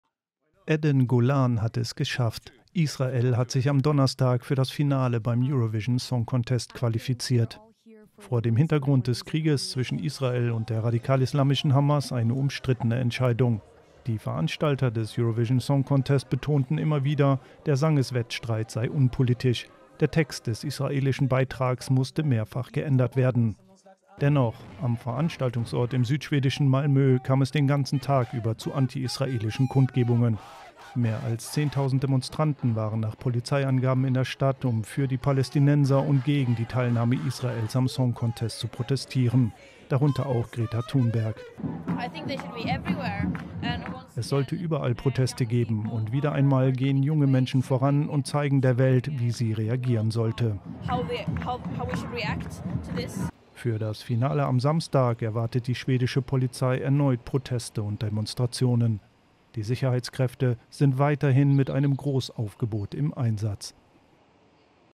Tausende Menschen nehmen vor Eurovision Song Contest an antiisraelischen Protesten teil
Vor dem Beginn des ESC in Malmö fanden Demonstrationen gegen die Teilnahme Israels statt. Die Veranstalter betonen, der Wettbewerb sei unpolitisch.

Bildrechte Thumbnail: TT News Agency/Johan Nilsson/Reuters